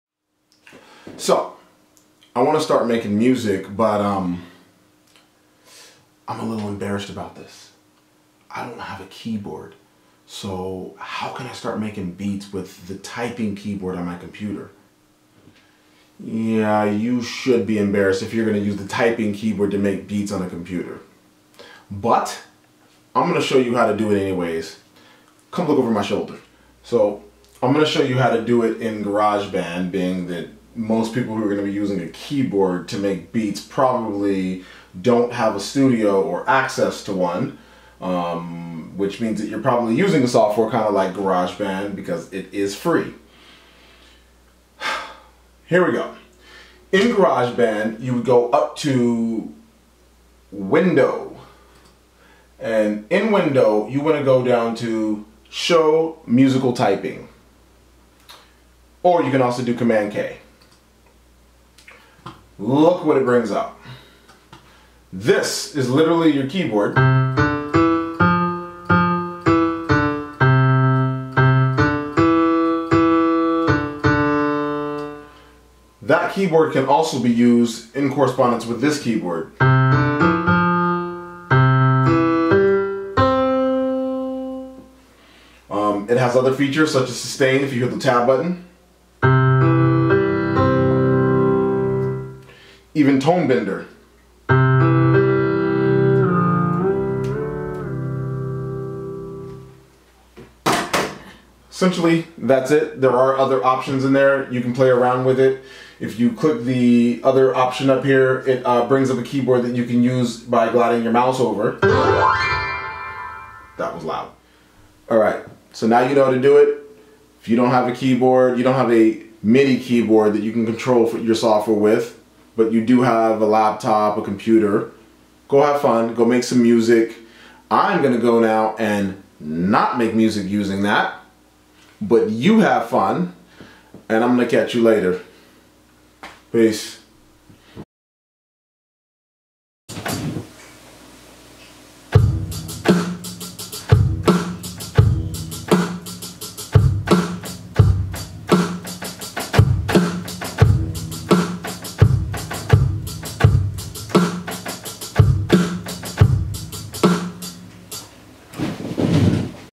Making Music Without a Midi Keyboard
Making Music Without a Midi Keyboard. Teaching beat making / music making... But this time without a midi controller or musical keyboard. You will learn how to use the alphanumeric typing keyboard on your computer or laptop.